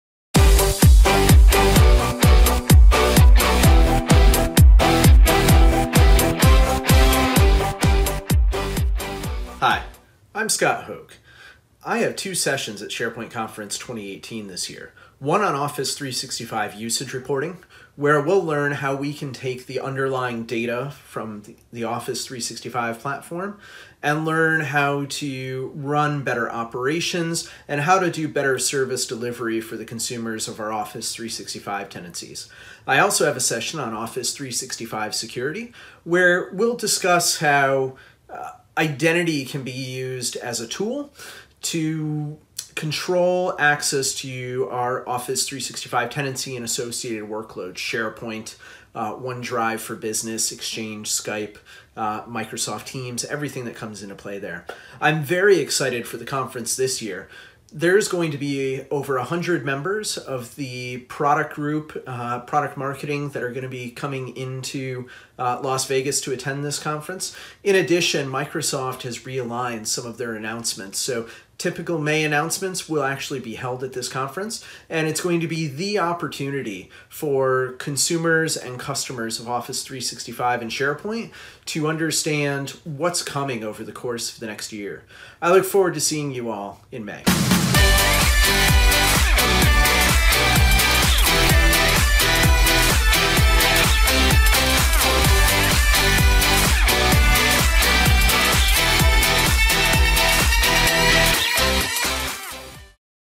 SharePoint Conference North America (@SPConf) Speaker Spotlight on Scott Hoag
SharePoint Conference North America (SPCNA - SharePointNA.com) is just around the corner from May 19th to 24th 2018 in Las Vegas, Nevada. Featured presenter, Scott Hoag, Cloud Solutions Architect at ACTS, shares what you can expect at SPCNA @SPConf